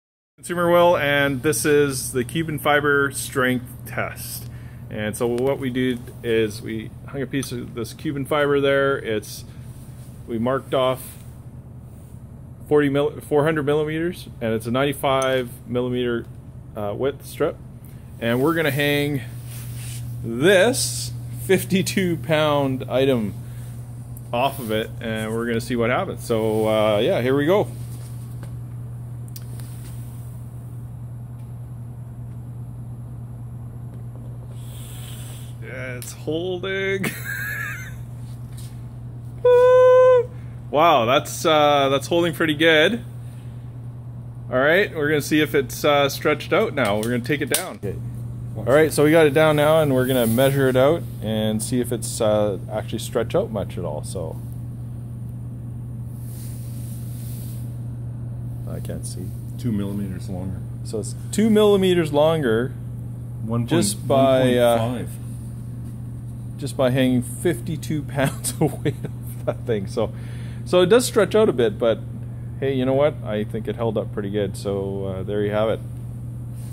Dyneema (Cuben) Fiber Strength & Stretch Test
Testing how strong Cuben Fiber is and how much stretch it has.